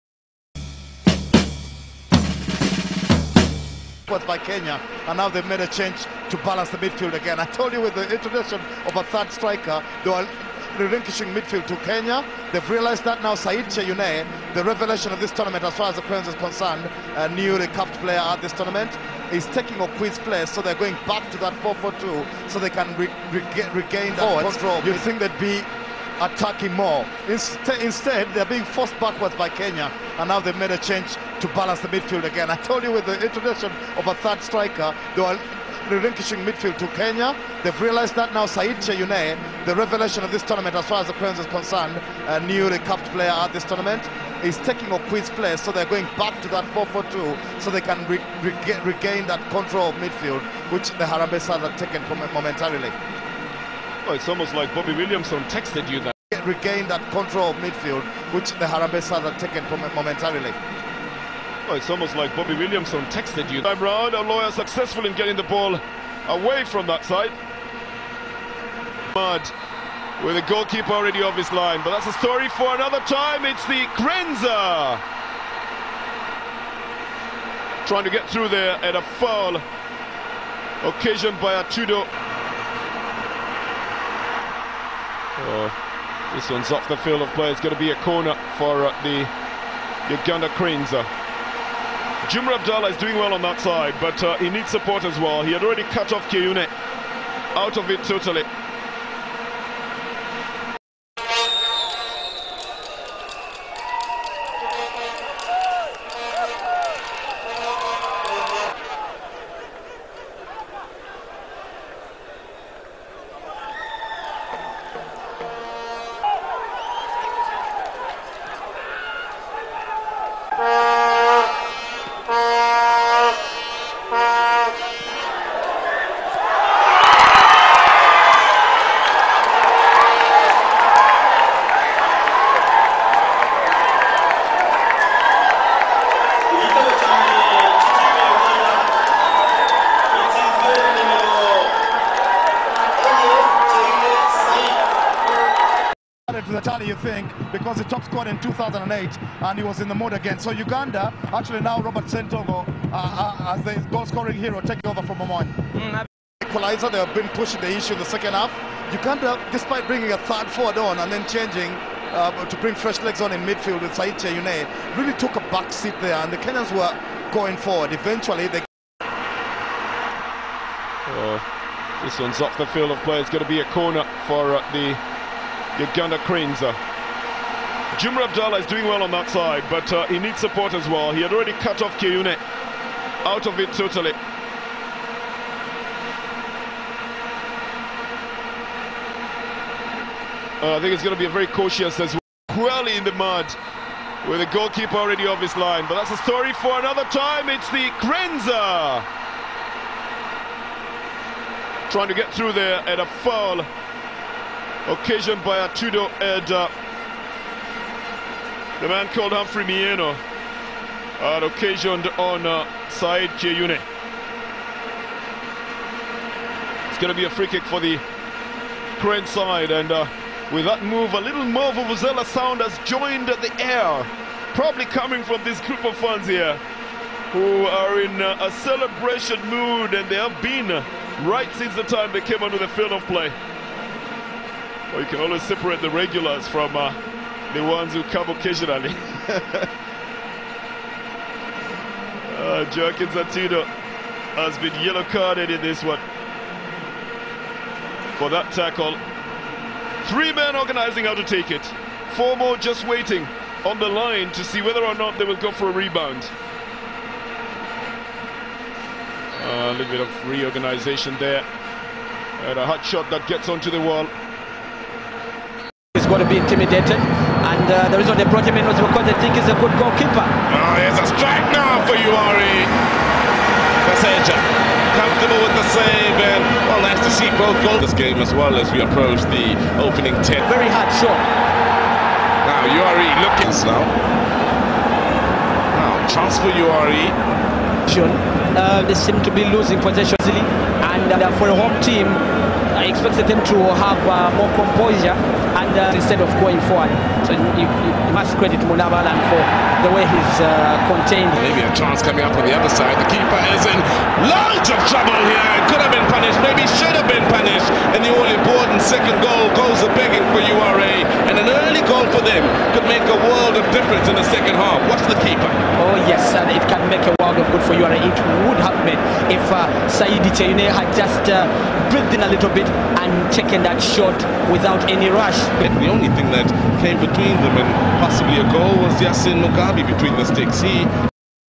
Saidi Kyeyune profile
The Video relays Video profile of the Ugandan upcoming midfielder general who has made his international appearance to Senior National team at age of 19 in 2012.

Saidi Kyeyune plays his club football with URA SC and he joined the club in 2011/2012 season. He has assisted the club to win one Cup (FA) and super Cup.

Thoug mid fielder Saidi has scored a total of 12 goals for in all matches within the two seasons